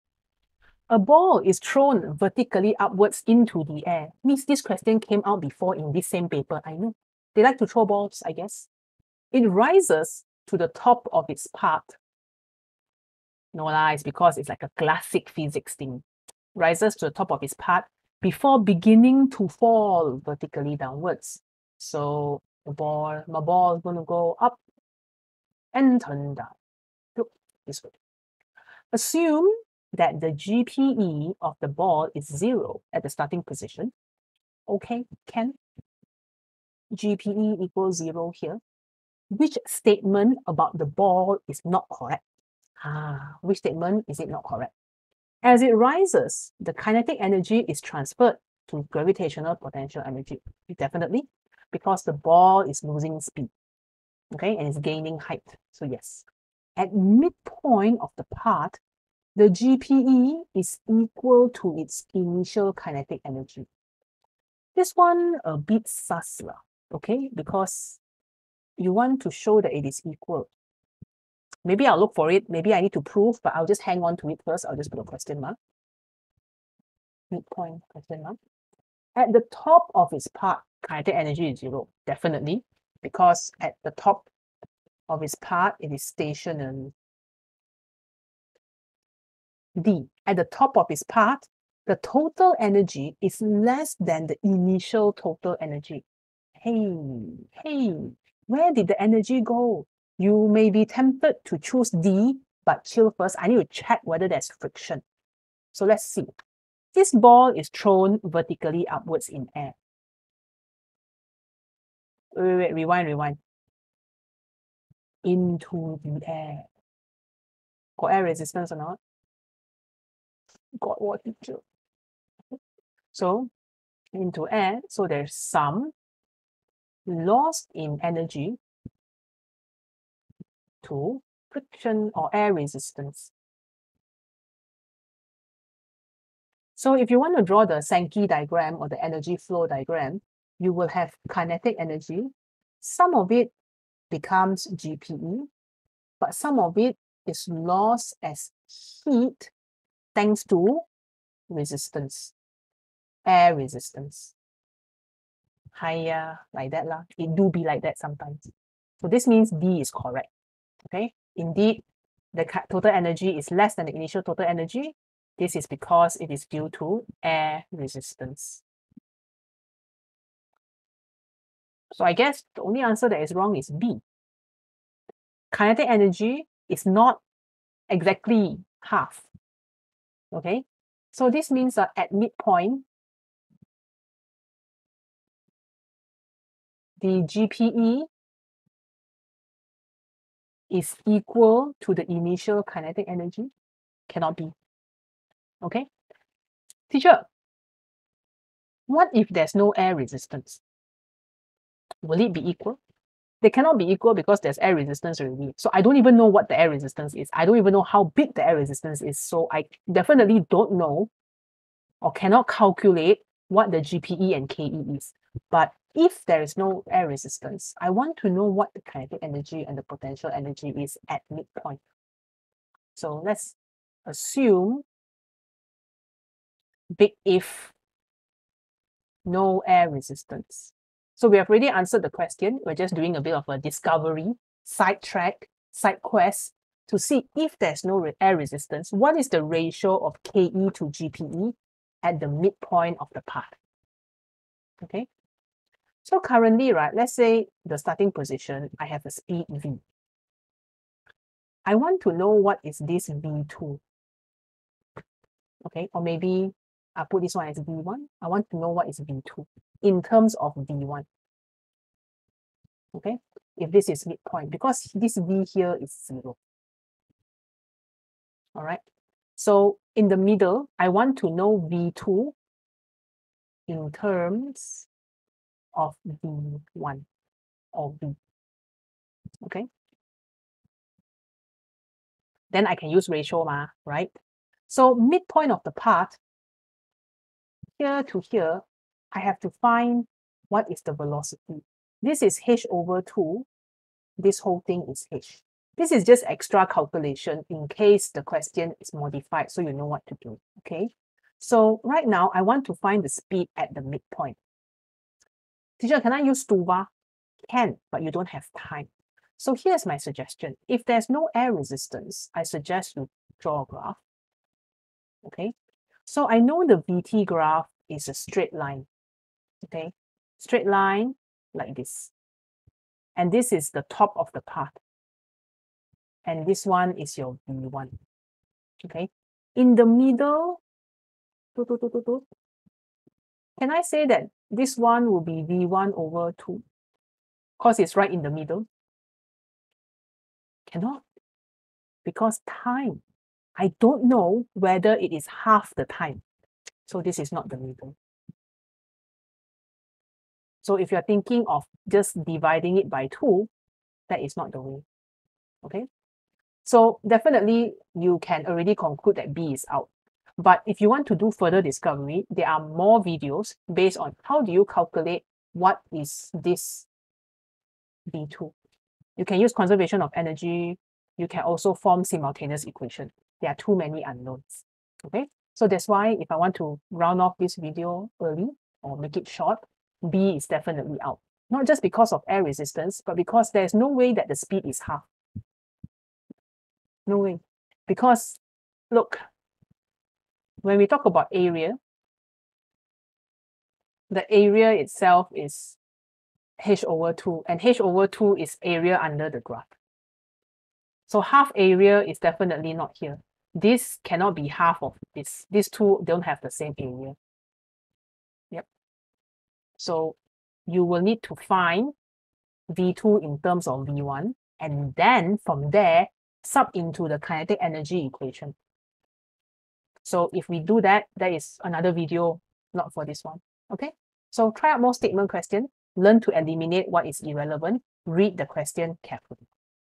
ON21 P12 Q16 Energy Statements about Ball | Oct/Nov 2021 | Cambridge A Level 9702 Physics
9702/12/O/N/21: A ball is thrown vertically upwards into the air. It rises to the top of its path before beginning to fall vertically downwards. Assume that the gravitational potential energy of the ball is zero at its starting position. Which statement about the ball is not correct?